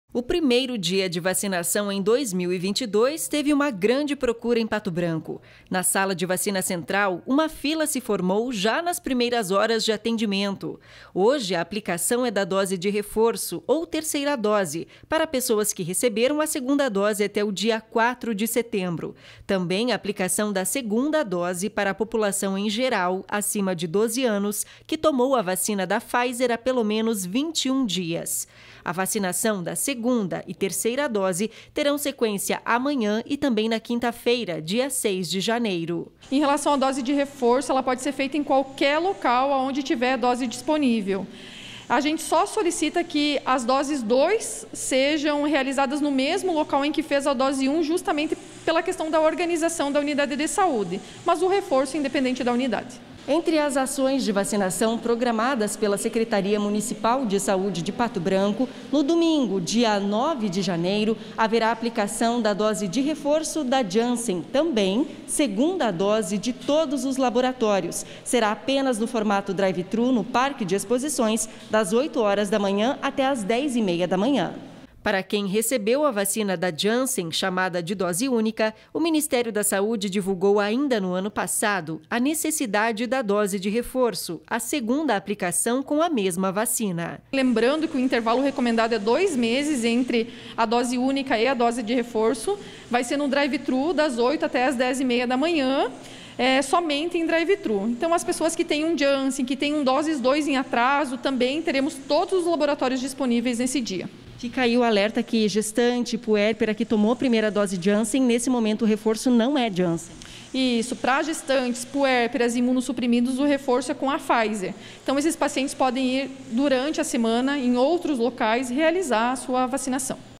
Retomada da vacinação em Pato Branco
Saúde: Em Pato Branco hoje (04/01) foi o primeiro dia do ano de vacinação contra a Covid 19.